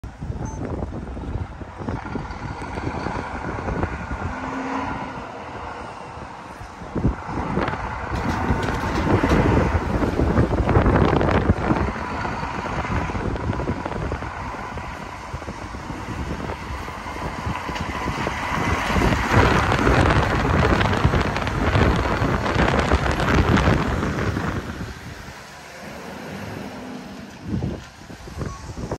VOLVO FH LTMG1800 旋回体輸送 NICHIUN
NICHIUNさんのLTMG1800の旋回体をVOLVO FH 6×4が牽く3軸の中低床台車に載せて夜間に輸送していきます。